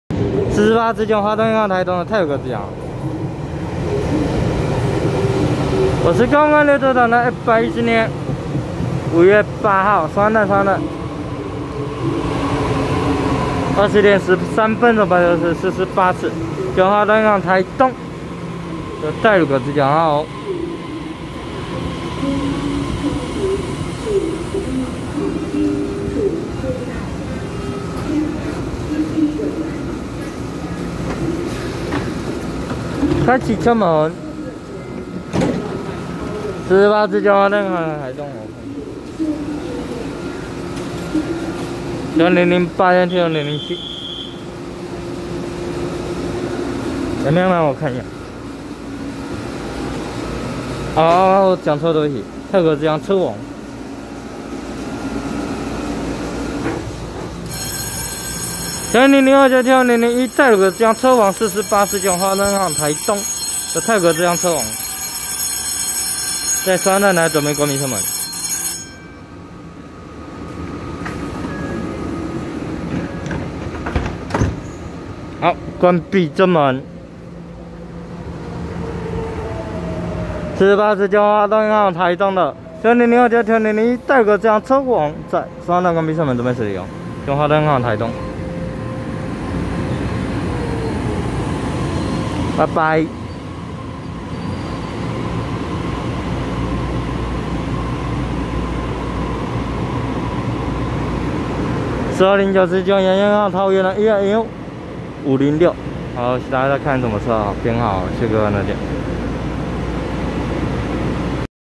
448次經由花東線開往台東的TEMU1002+TEMU1001車王太魯閣自強號，和4209次經由宜蘭線開往桃園的EMU513+EMU506區間車
110/5/8松山站20:13分拍攝448次經由花東線開往台東的TEMU1002+TEMU1001車王太魯閣自強號，和4209次經由宜蘭線開往桃園的EMU513+EMU506區間車